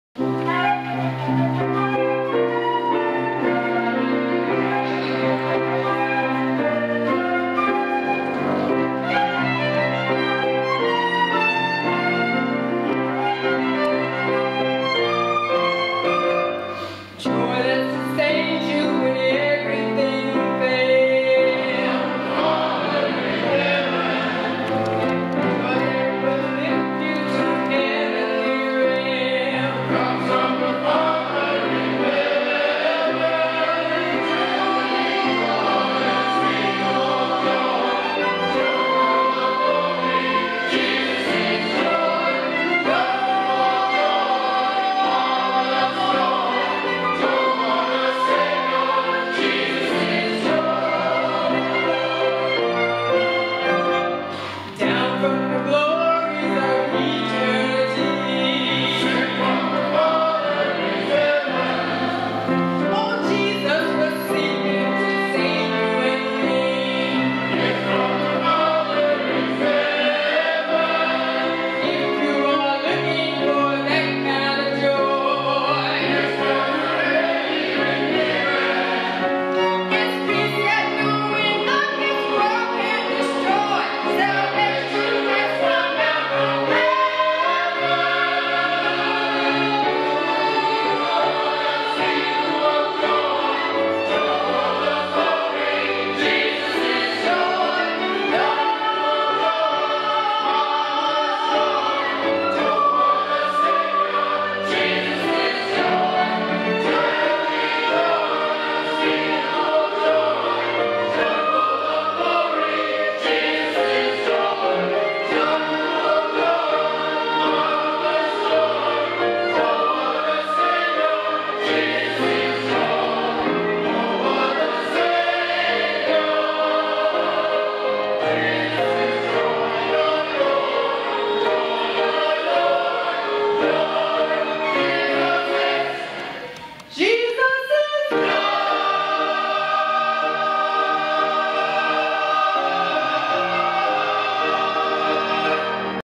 Carthage Cantata | Christmas Cantata North Carolina | Carthage United Methodist Church
Every Christmas Carthage United Methodist Church celebrates the birth of Christ with a beautiful Cantata. Directed by Choir leader Edward Lee the choirs of UMC Carthage and Mount Zion AME join to create a remarkable event. Augmented by extremely talented musicians this event is a must GO for everyone who loves music and the Christmas Message.